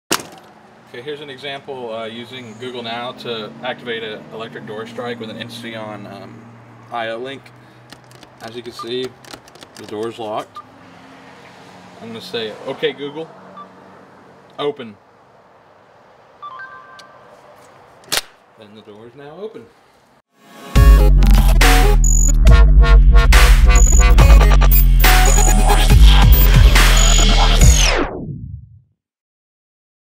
Google Now Home Automation
Opening an electric door strike using Google Now search API, Autovoice, Tasker, Homeseer, & Insteon home automation.